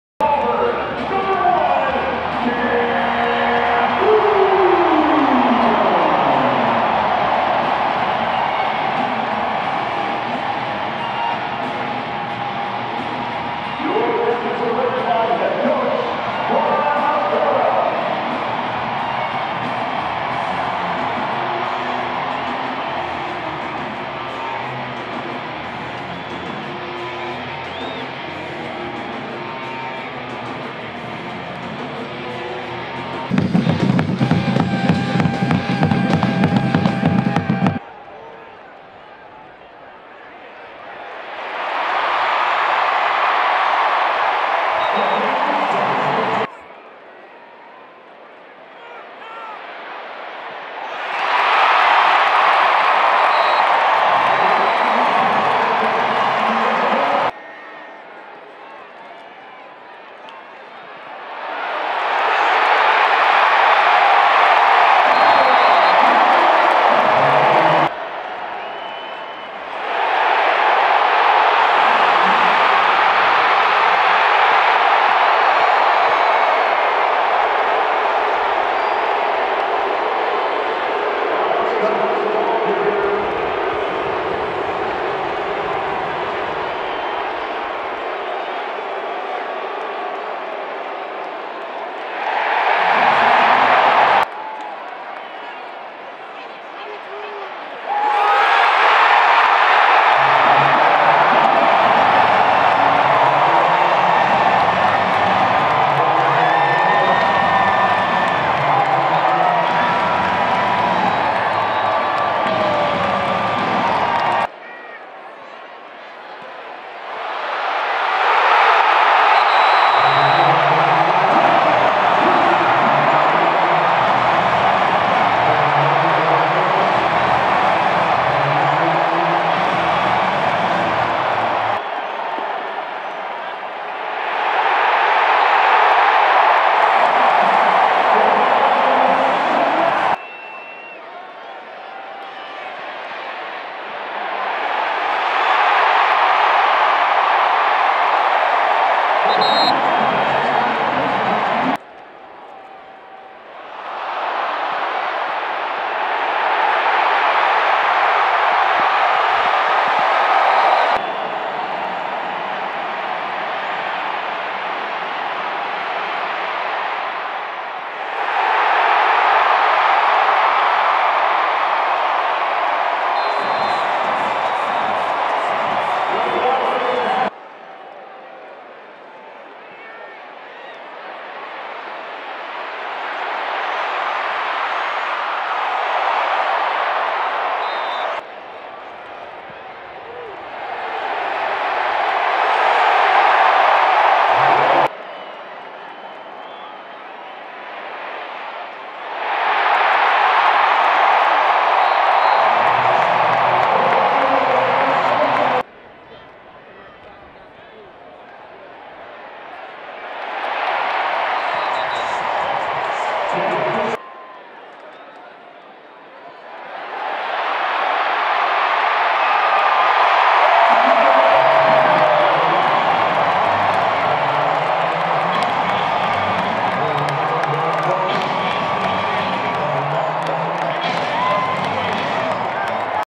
Sounds from the Stadium Ravens vs Panthers (2018)
Some of the best sights and sounds from the field at Bank of America Stadium.

Ravens vs Panthers